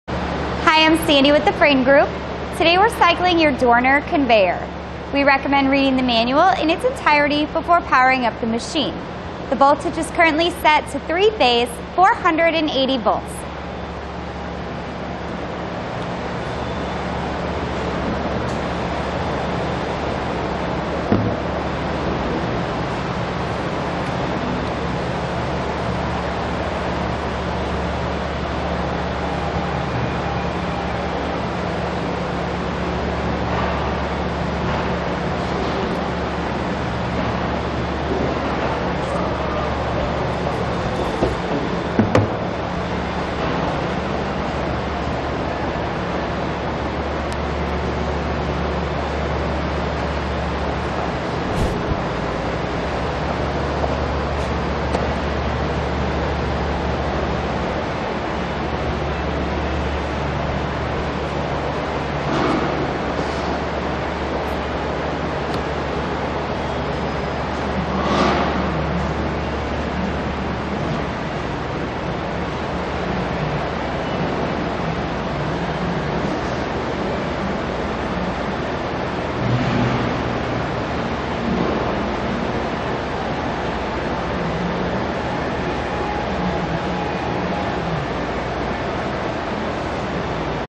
Dorner Model 3100 18" W x 132" L Belt Conveyor Demonstration
Dorner, Model 3100, 18" wide x 132" long belt conveyor. 18" wide and 132" long rubber belt has an infeed / discharge range of 30" - 35". Equipped with a ½ hp drive and mounted on (4) leveling legs.